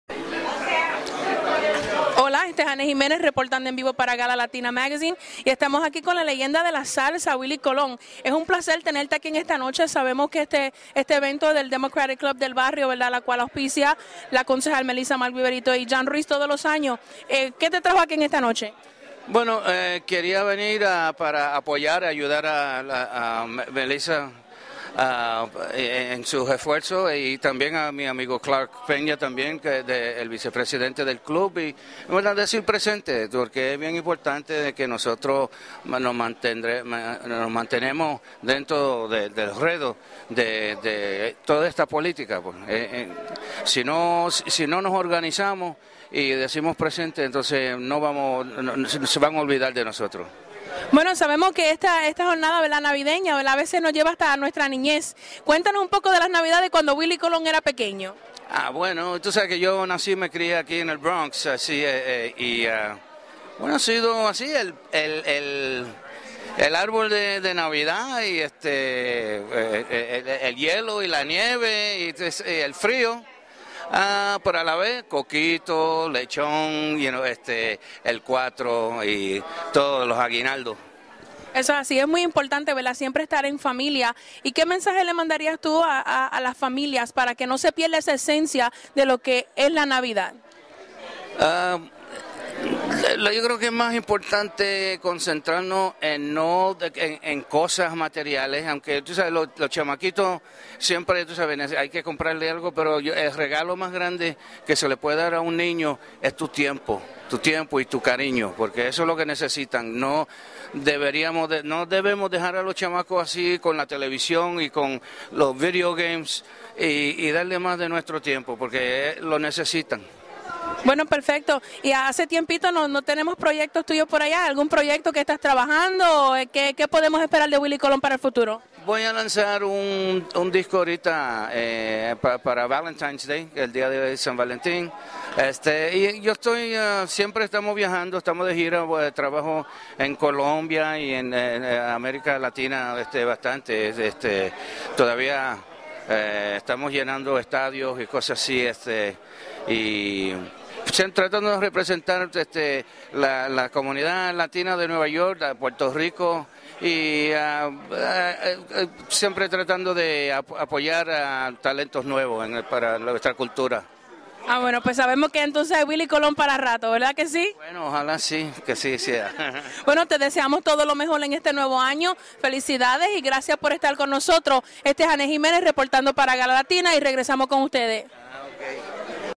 Willie Colón Entrevista con Annette Jimenez
Harlem Democratic Club Christmas Party 2013 - Thrown by Clark Peña